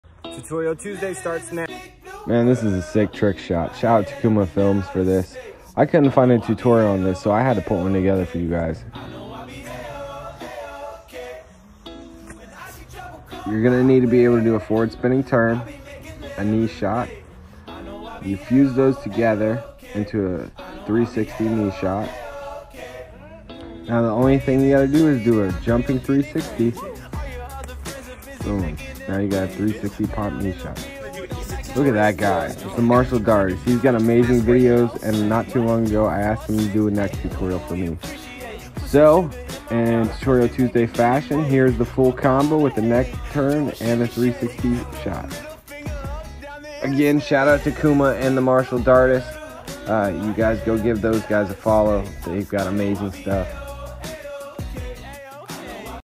Rope dart pop 360 knee shot tutorial trickshot ropedart @kumafilms @martialdartists begin
This week I will be teaching a pop 360 knee shot as well as featuring fellow tik toker the martialdartist, a rope dart master mind. I will be taking his move and fusing it with my move in a full combo